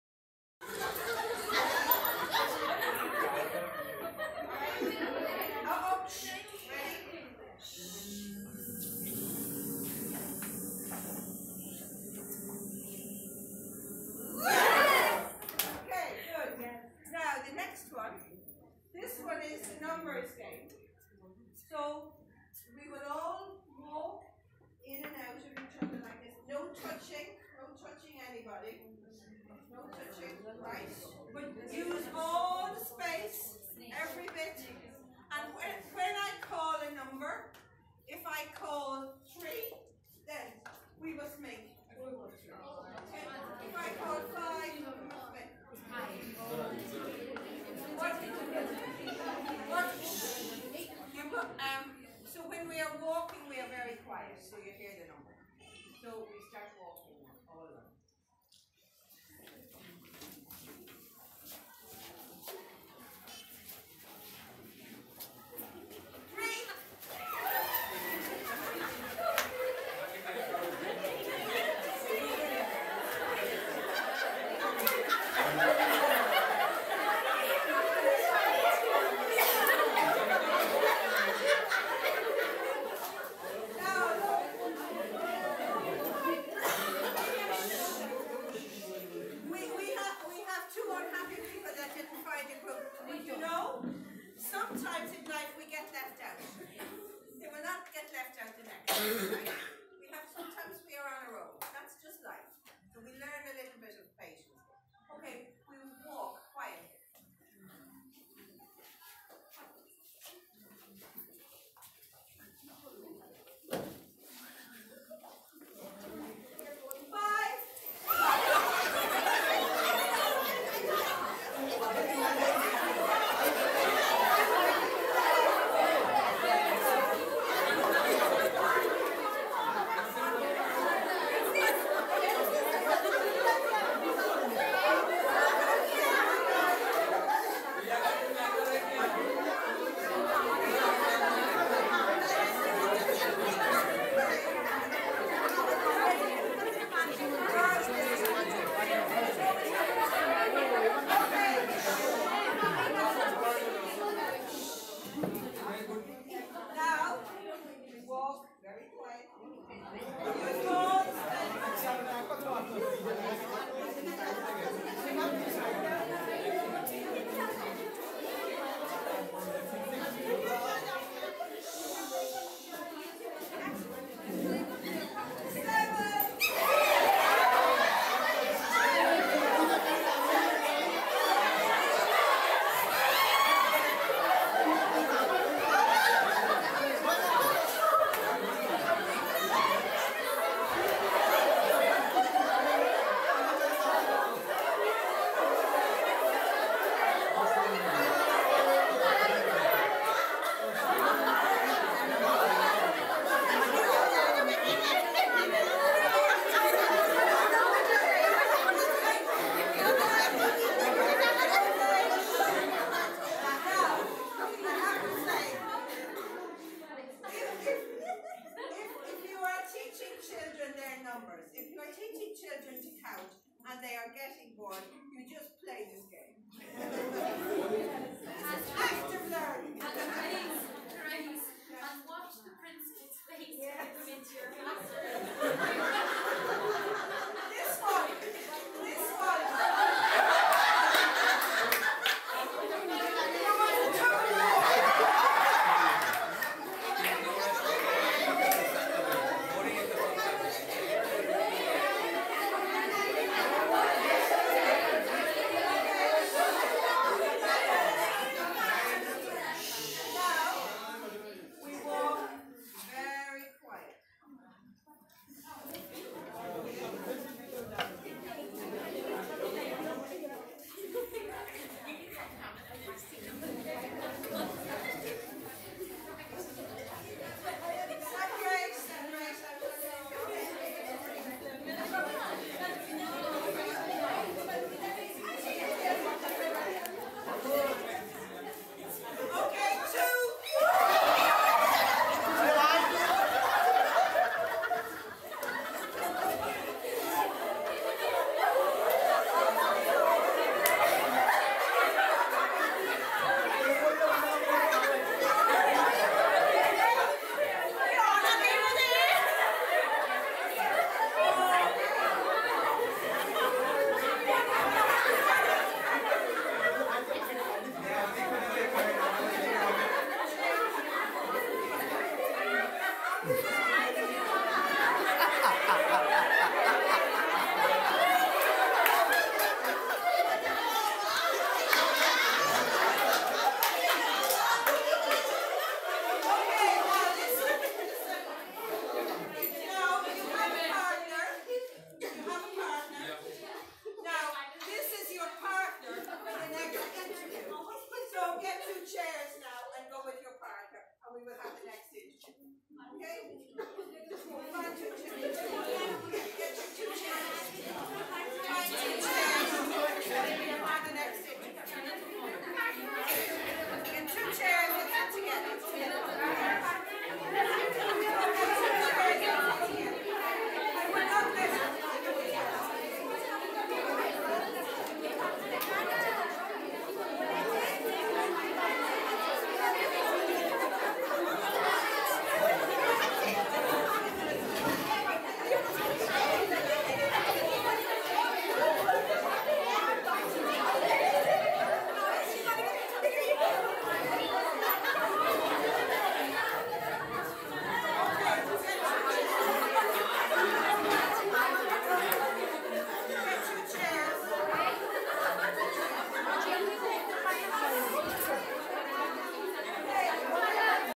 Joyful teaching methodology by Froebel college , Ireland
Teachers are learning different playful methodology for creating joy learning in government school